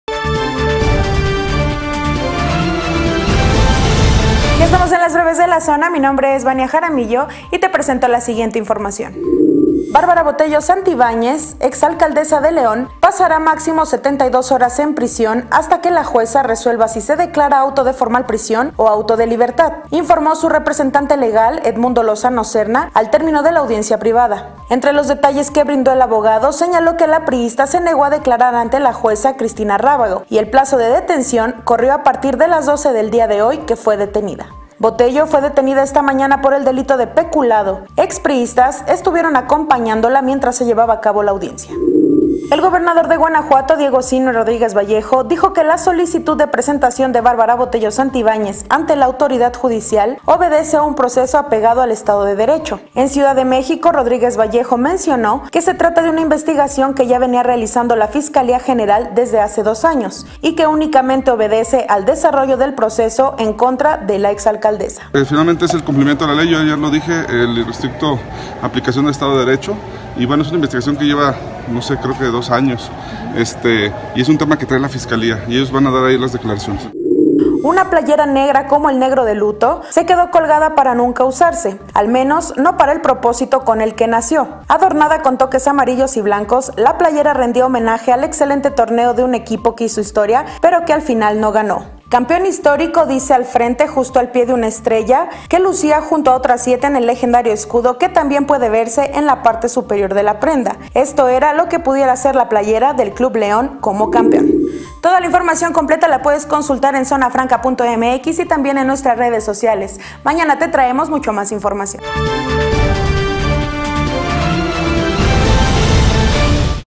Breves de la Zona 30 de mayo 2019 emisión nocturna
Hoy en las BrevesDeLaZona con Vania Jaramillo
- Ediles del PAN niegan falta de vigilancia en la Tesorería de León; ‘es triste que los funcionarios roben’: síndica.
- Violencia política de género tras llamada, acusa Alma Alcaraz Hernández.
- Ciudadanos se manifestaron en instalaciones de Sapal; exigen cuota única.

Esto y más lo encuentras en zonafranca.mx